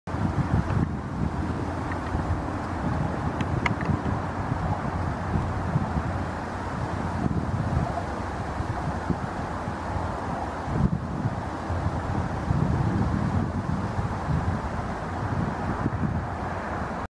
Red Squirrel climbing the tree
Eurasian Red Squirrel (Sciurus vulgaris) climbing the tree.

Orava (Sciurus vulgaris) kiipeää koivuun.